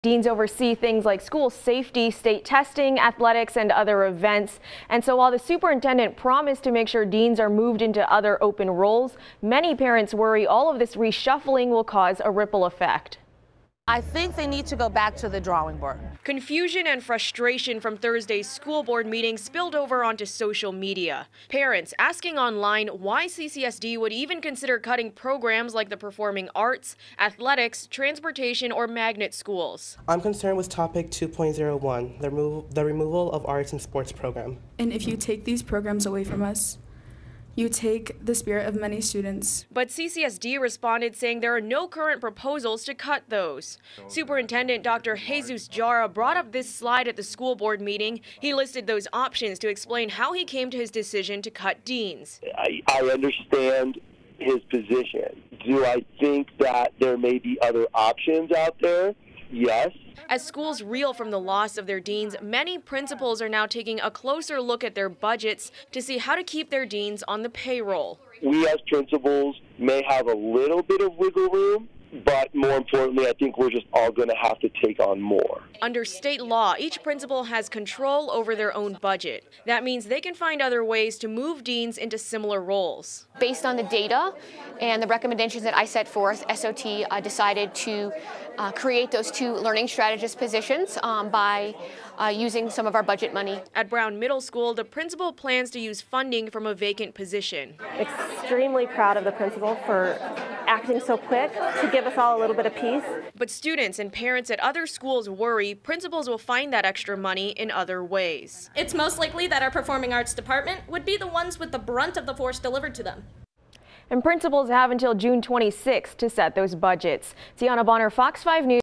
What happens next for Las Vegas schools
After CCSD announced they would cut all dean positions, here is what happens next for Las Vegas schools.